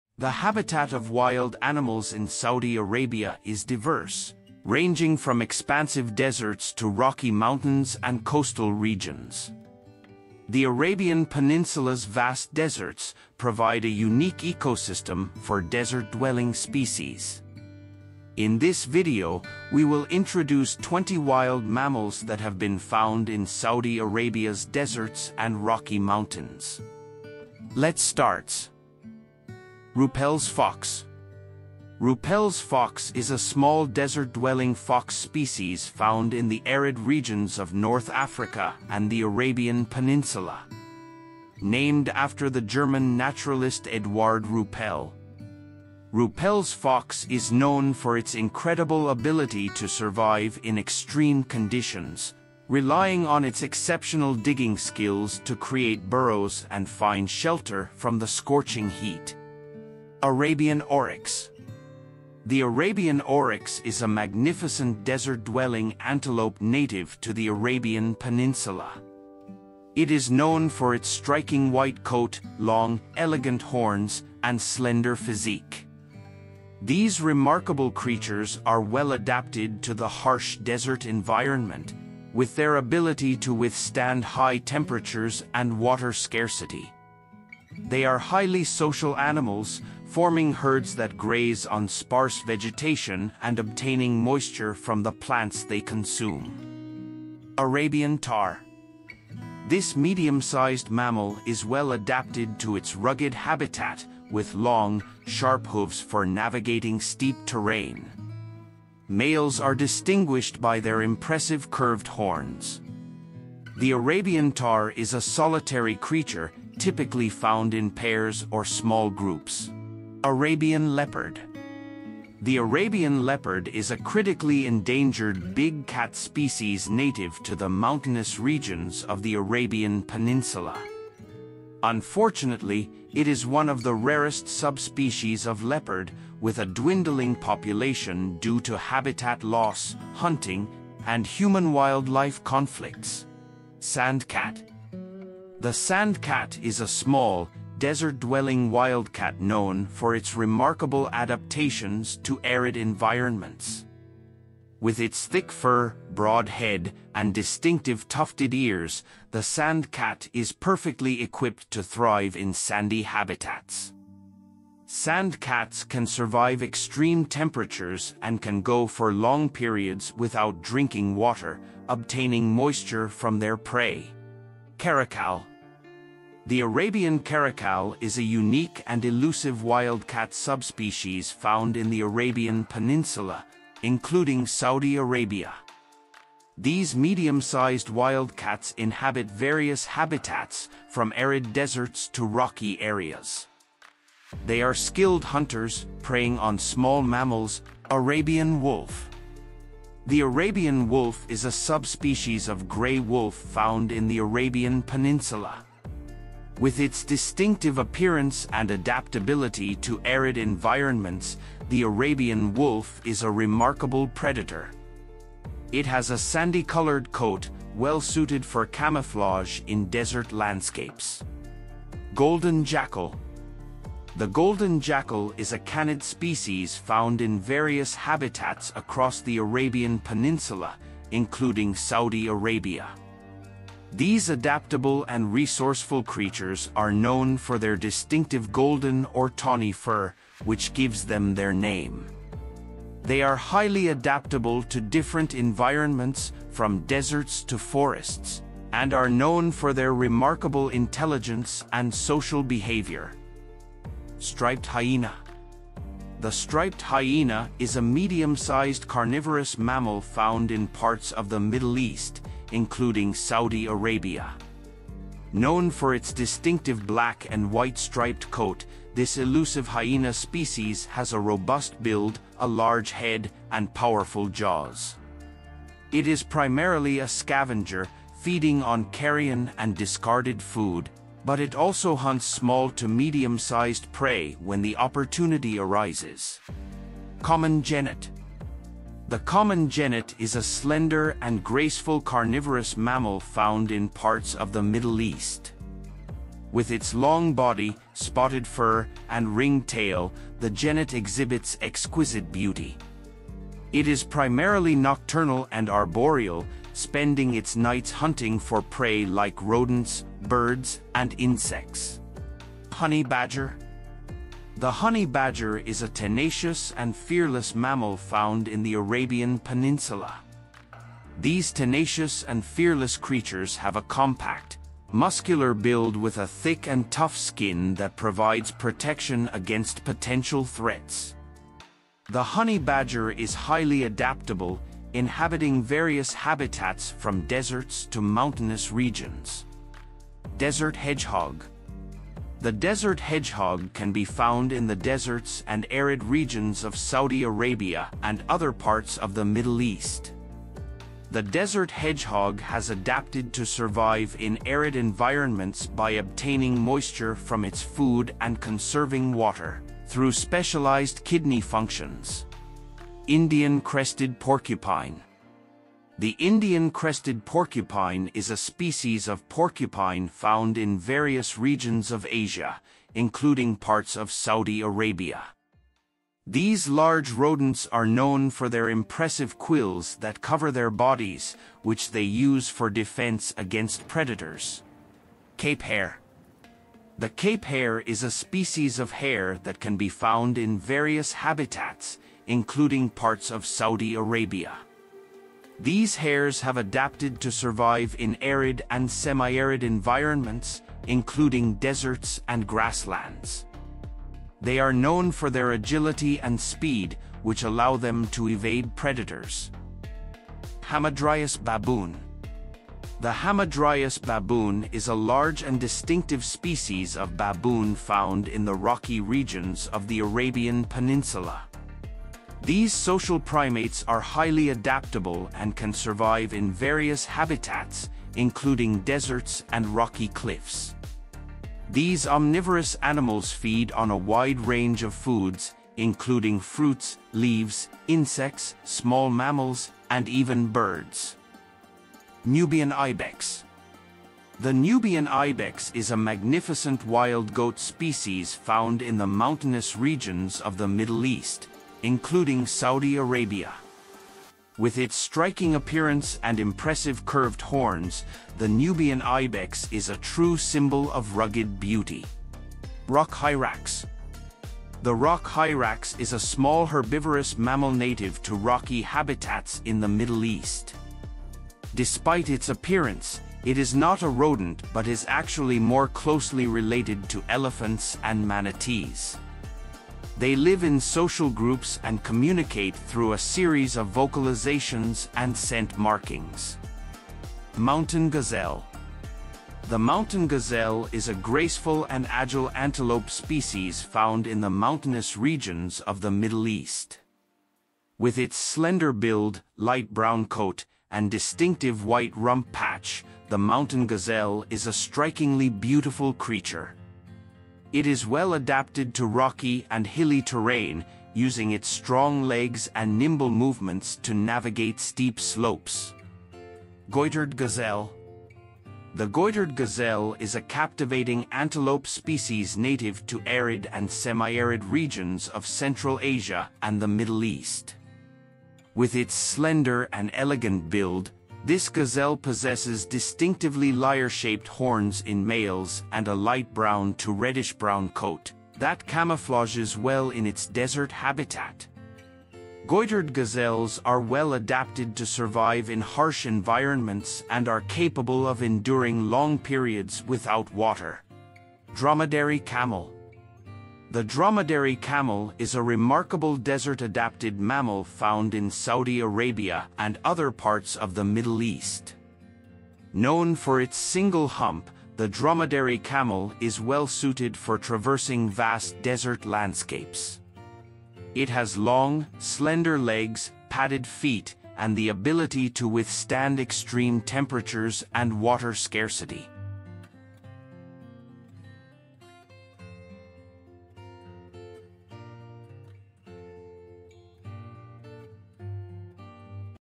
Arabian Wildlife- 20 Surprising Wild Mammals living in the Saudi Arabian Desert
The wildlife of Arabia is completely different from the wildlife of other countries. Because it is very difficult for any animal to survive in the Arabian desert or arabian rocky mountains. Despite the harsh climate, several animals survive in the desert environment of Arabia. In today's video we will introduce you to 20 mammals that survive the harsh climate of Arabia. Hope you like it.
desert animals for kids / animals in the desert